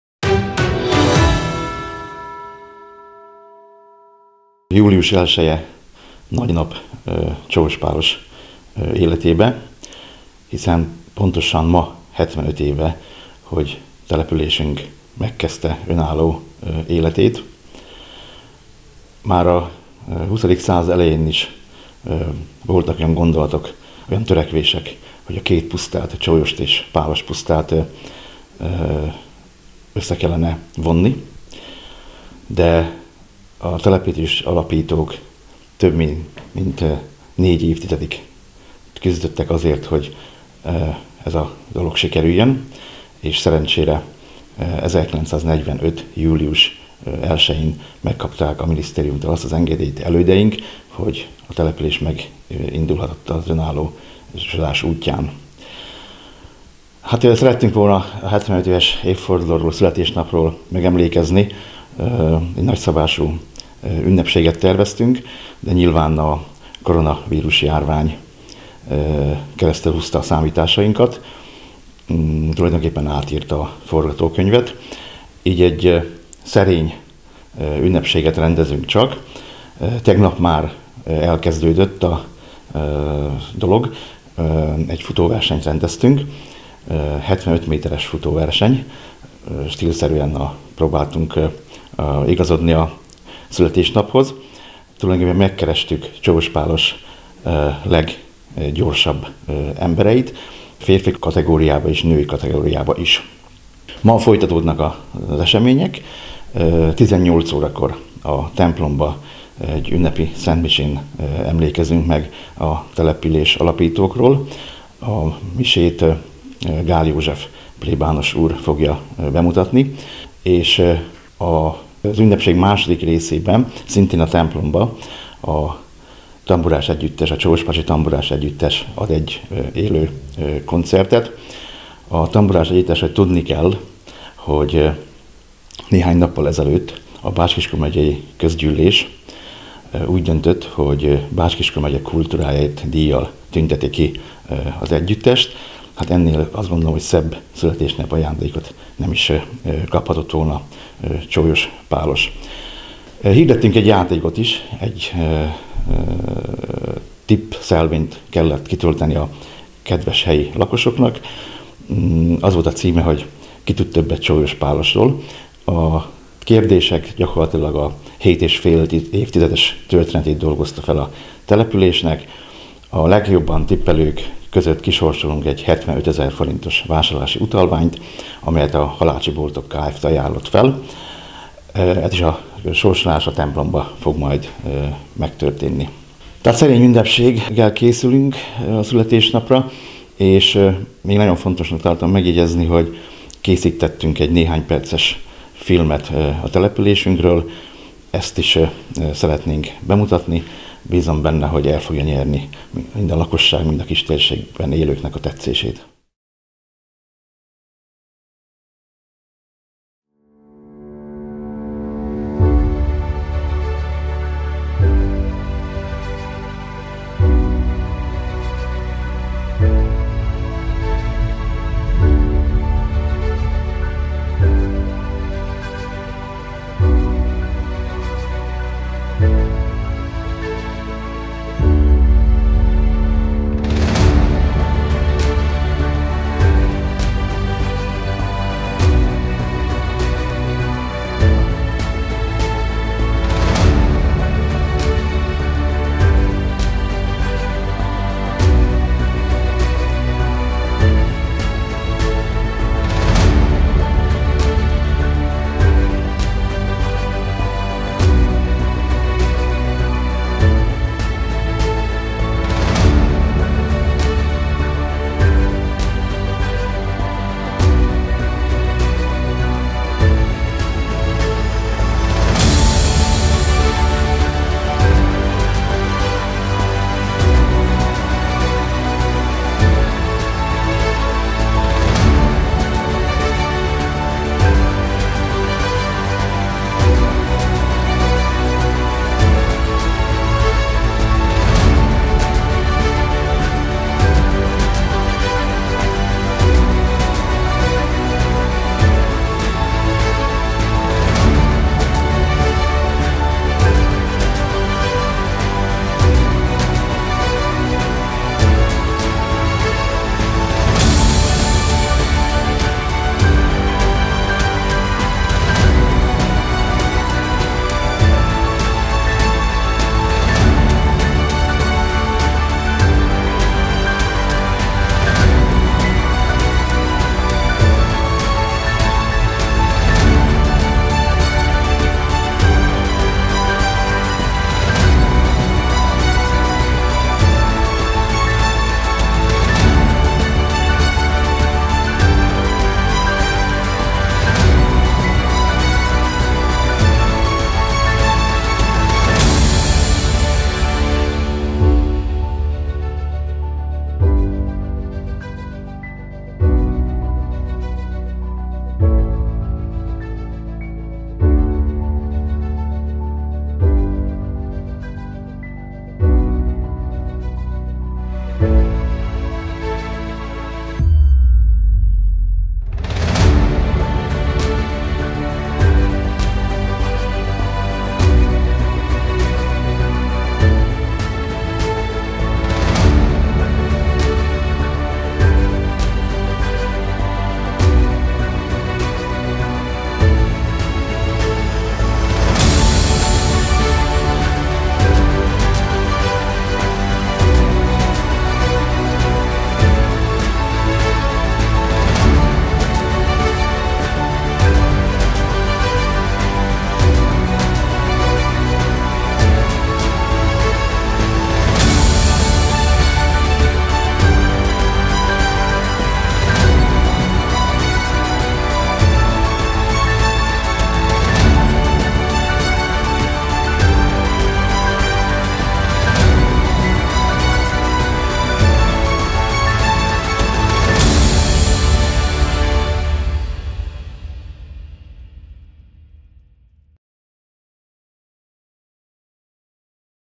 IMÁZSFILM KÉSZÜLT CSÓLYOSPÁLOS 75 ÉVES ÉVFORDULÓJÁRA
Az 1800-as évek második felében felosztott Pálos és Csólyos puszták igen gyorsan benépesültek. „1900-ban Csólyoson 1188 fő, Páloson 1234 fő lakott” – olvasható a Fodor Ferenc szerkesztette; Tanulmányok Csólyospálos történetéről és népéletéről című, 1995-ben készült kiadványban. A két puszta egyesítését és önálló községgé alakulását már 1903-ban, majd később is szorgalmazták az ott élők, de utána a községgé válás 1910 után hosszú időre lekerült a napirendről. Az önálló községgé alakulást 1940-ben a belügyminiszter is javasolta, ám a háború közbeszólt.
A Kiskunfélegyháza járásba kebelezett Csólyospuszta és a Kiskunmajsához csatolt Pálospuszta lakossága 1945. május 12-én ismét kérelmezte belügyminisztériumnál a két puszta egyesítését. Az ideiglenes nemzeti kormány belügyminisztere, Erdei Ferenc végül engedélyezte az önálló településsé alakulást.
A Csólyospálos néven egyesült két puszta 1945. július 1-jén kezdte meg önálló életét. A helyi képviselő-testület 1945. július 22-én tartotta alakuló ülését. „Az Isten nevében és a jó Isten áldását kérjük községünk képviselő-testületének munkájára, hogy abból a közre áldás, községünk lakosaira jólét, virágzás, béke, szeretet és megelégedés származzon” – így kezdődik az ülés jegyzőkönyve.
A születésnapról egy nagyobb ünnepség keretében szerettek volna megemlékezni, de a koronavírus-járvány közbeszólt. Á. Fúrús János polgárnester elmondta, így szerényebb keretek között emlékezhetnek meg a jeles évfordulóról. A sportpályán kedden futóversenyt rendeztek, ahol 75 métert kellett teljesíteniük az indulóknak. Szerdán (július 1-jén) pedig ünnepi szentmise lesz a római katolikus templomban, majd a Csólyospálosi Tamburás Együttes ad koncertet, amely a napokban elnyerte a Bács-Kiskun Megye Kultúrájáért díjat. A polgármester elárulta, hogy készült egy rövidfilm településről, amit most mutatnak be a lakosságnak.